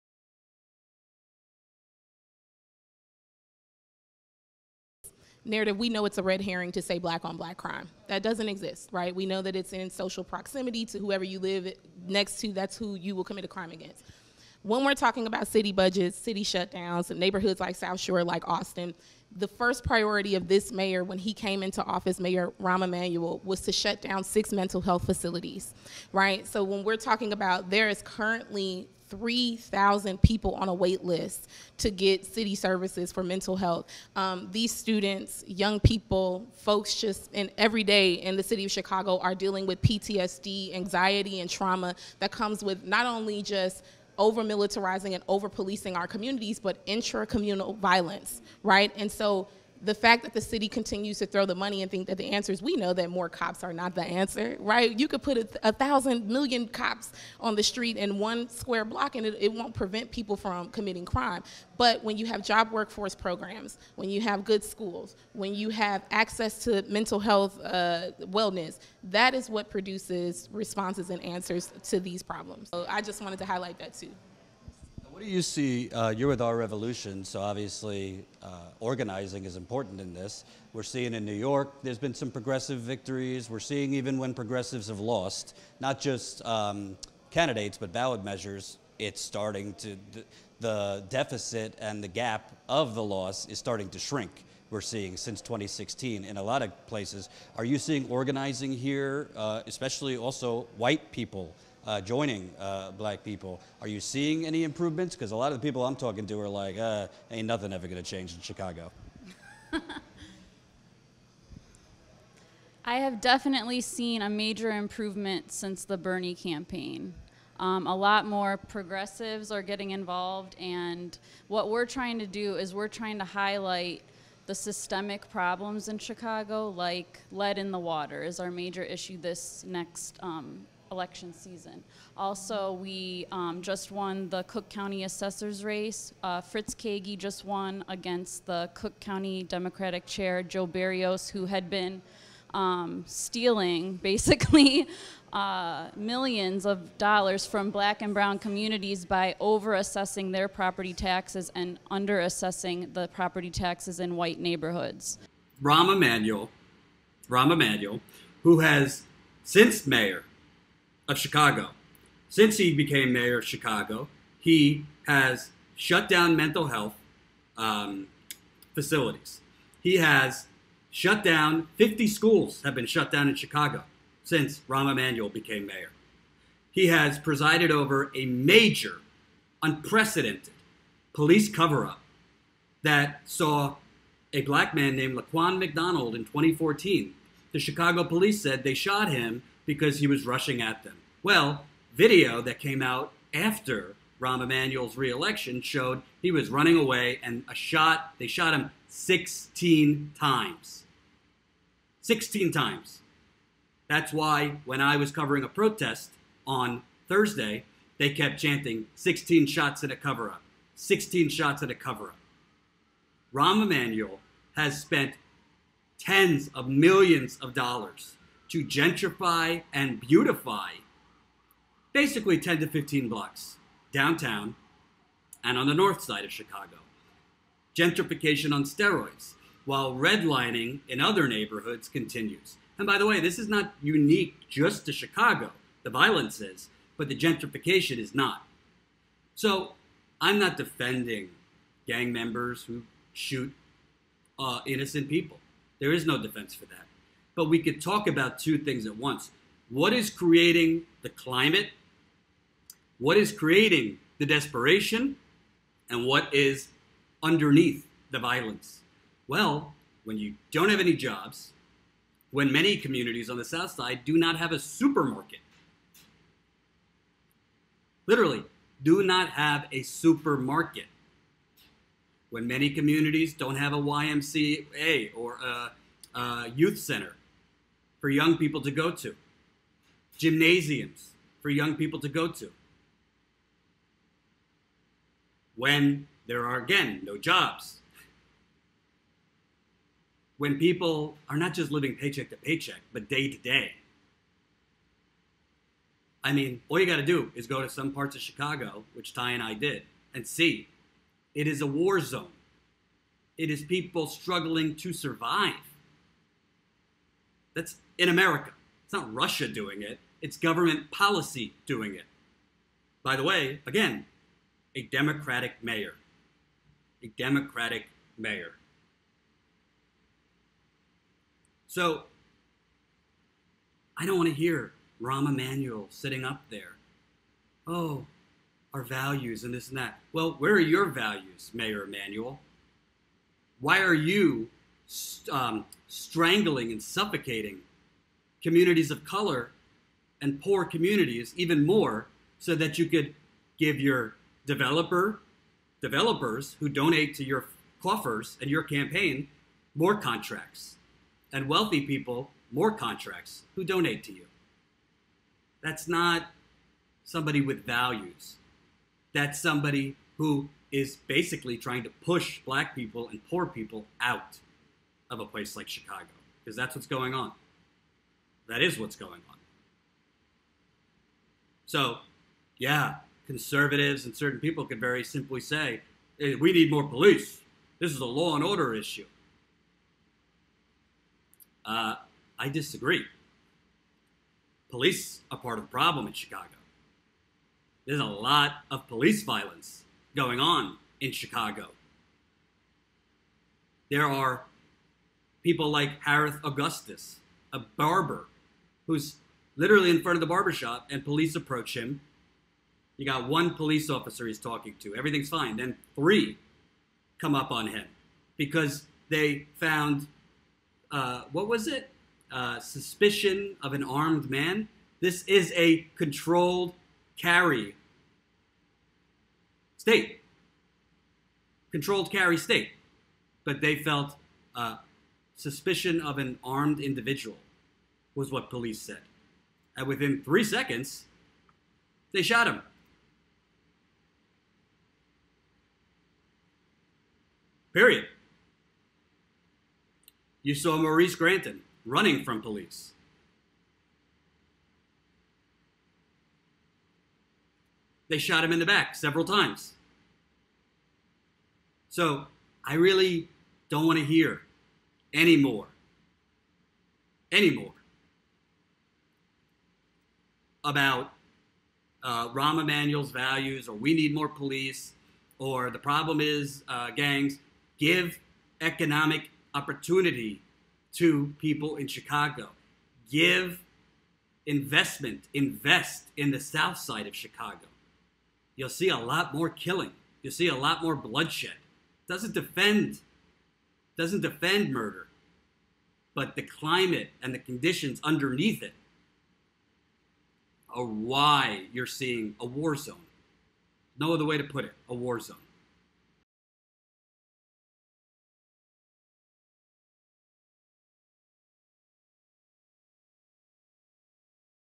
Chicago is a WAR ZONE That Rahm Emanuel is Allowing to Explode
Jordan spoke with Chicago organizers and activists in Chicago about the national Democratic Party doing little about the police violence against black people along with the economic despair. SUPPORT Jordan's Chicago reporting